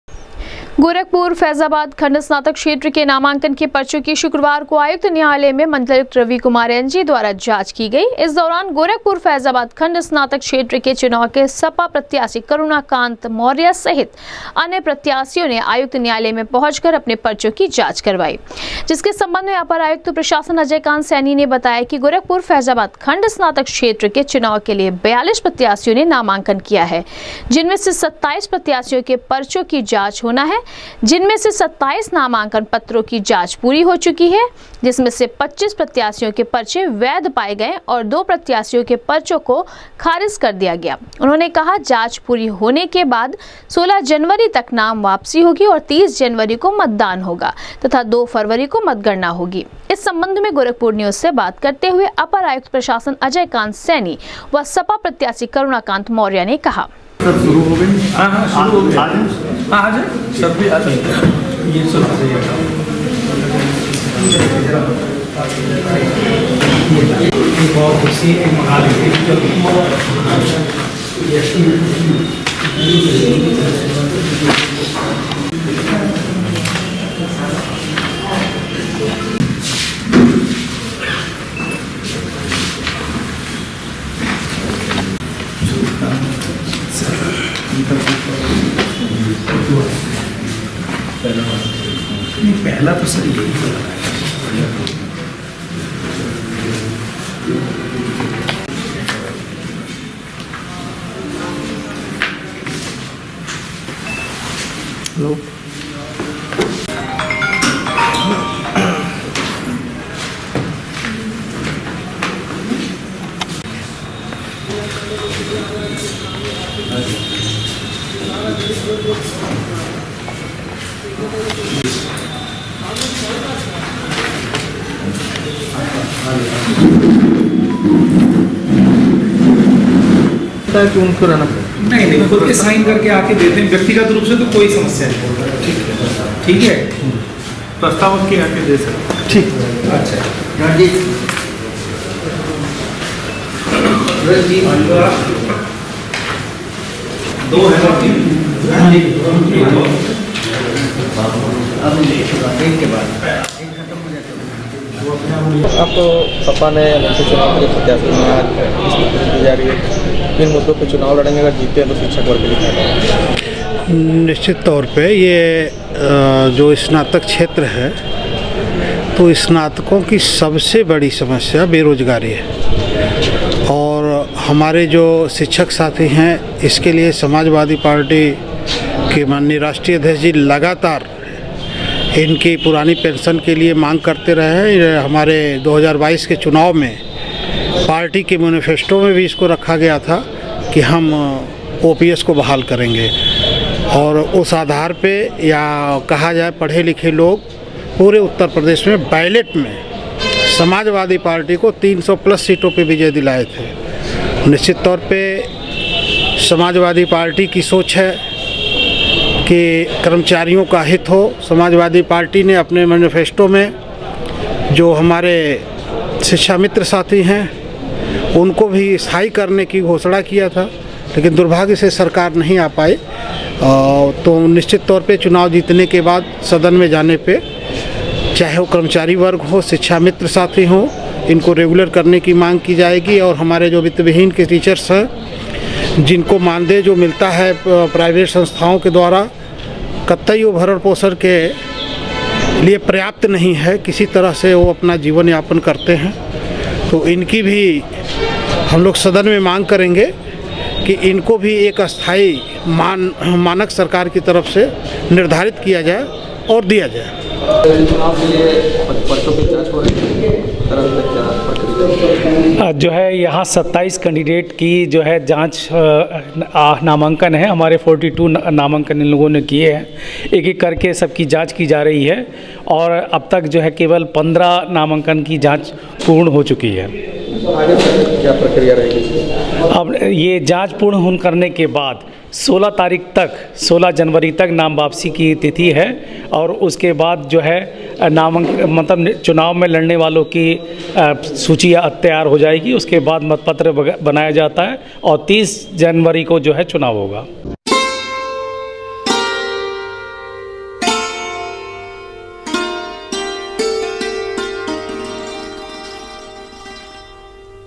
गोरखपुर फैजाबाद खंड स्नातक क्षेत्र के नामांकन के चुनाव के लिए दाखिल पर्चो की मण्डलायुक्त रवि कुमार
गोरखपुर फैजाबाद खंड स्नातक क्षेत्र के नामांकन के चुनाव के लिए दाखिल पर्चो की मण्डलायुक्त रवि कुमार एनजी ने की जांच,27 में से 25 प्रत्याशियों के पर्चे पाये गये वैध,दो पर्चो को किया गया खारिज,अपर आयुक्त प्रशासन अजय कांत सैनी ने दी जानकारी